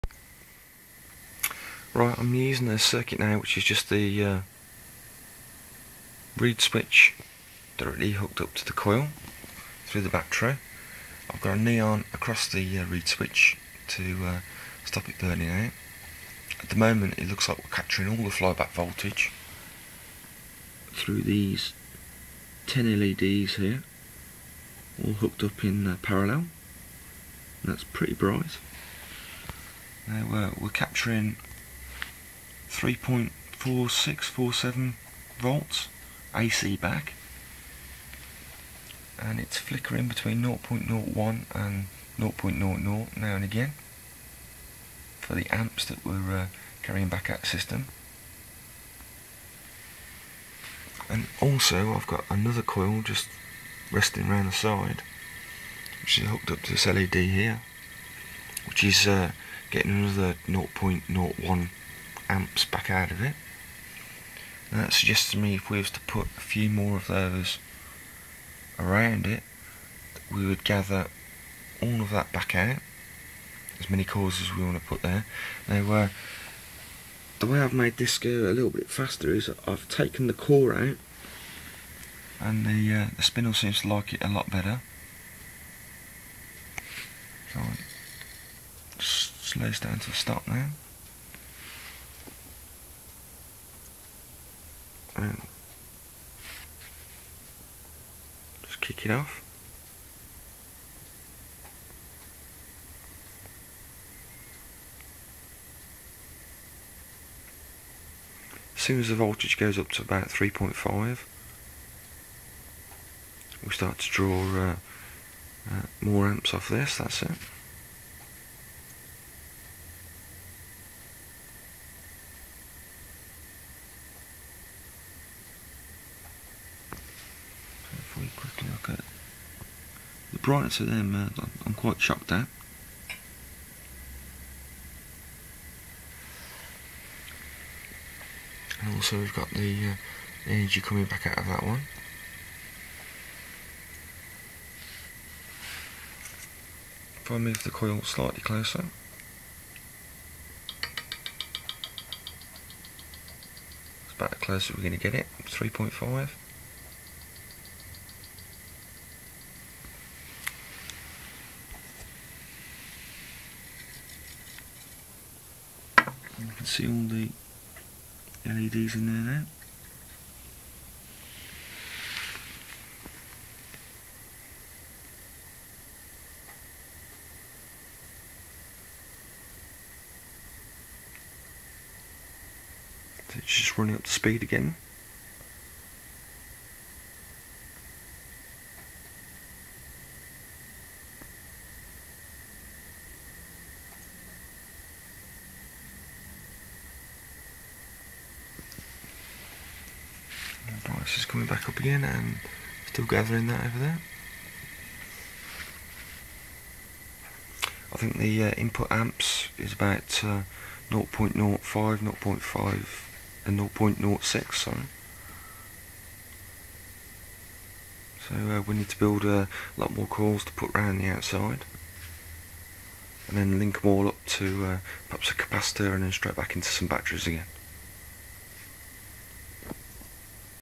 Pulse Motor (10 Led's Lit) 033
Lighting 10 Led's from the secondary Coil & 1 from an external generating coil